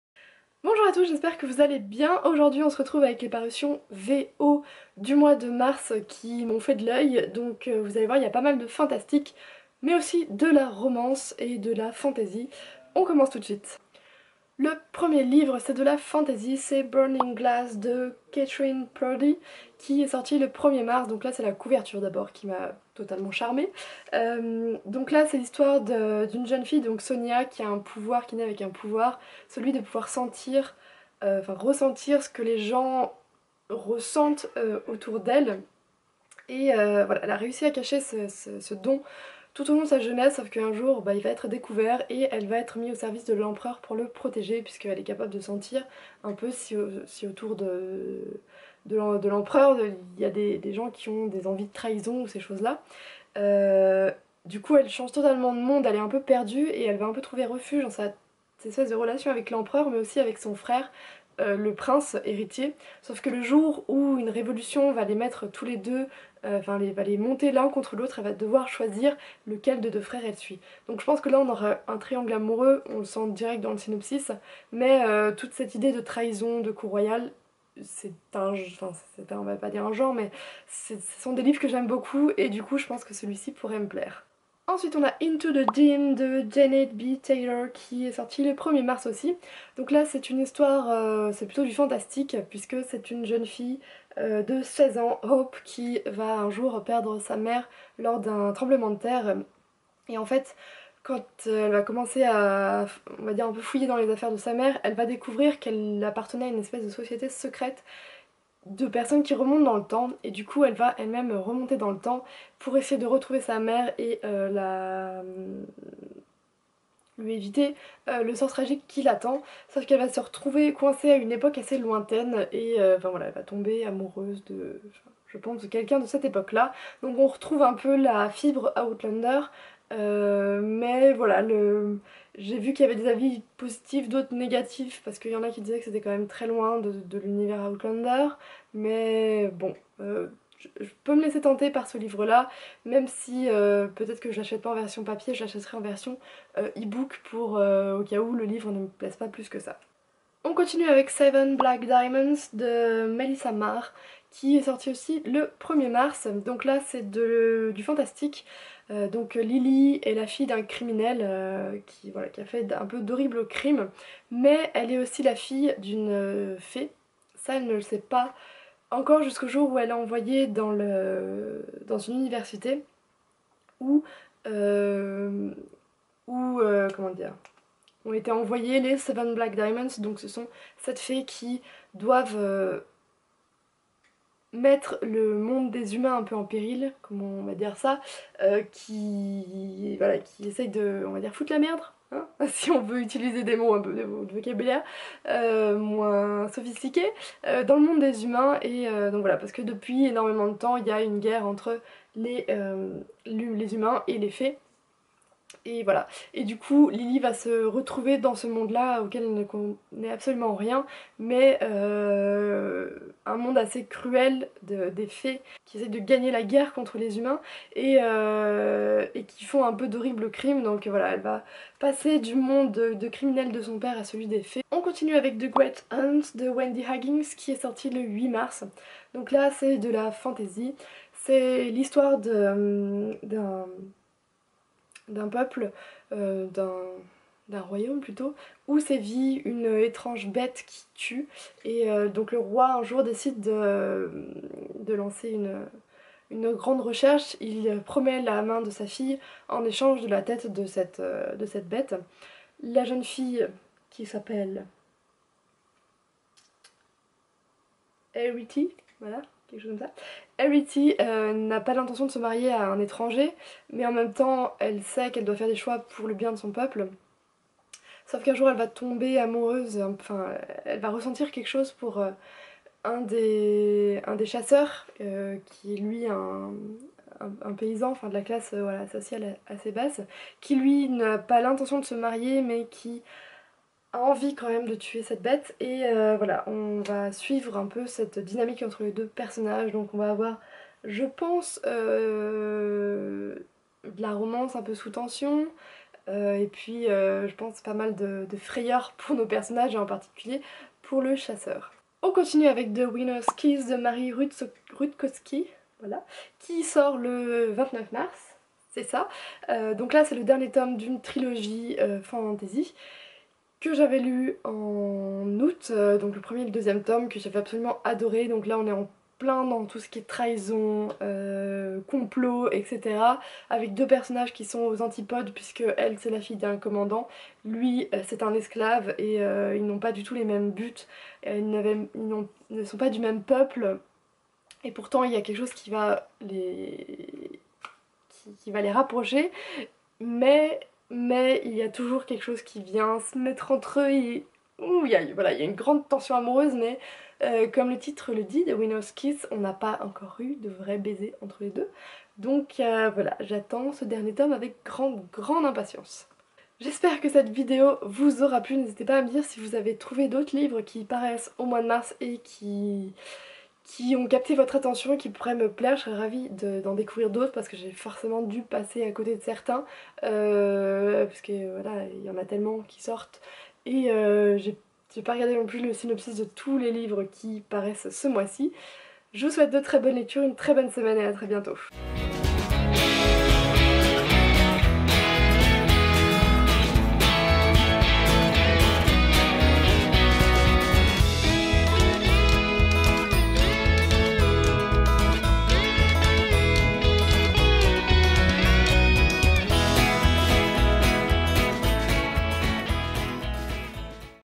Parutions VO Mars | Katlyn Purdie & Melissa Marr & Wendy Higgins
Découvrez les parutions VO qui m'ont fait de l'oeil en ce mois de Mars. Beaucoup de fantaisie avec bien sûr une pointe de romance. Vous trouverez par exemple le dernier Wendy Higgins ou encore Marie Rutkoski.

Livres cités:
- Burning Glass - Katlyn Purdie - Katherin Tegen Books
- Into the Dim - Janet B. Taylor - MHM Books
- Seven Black Diamonds - Melissa Marr - Harper Collin
- The Great Hung - Wendy Higgins - Harper Teens
- The Winner's Kiss - Marie Rutkoski - BYR

Où me trouver ?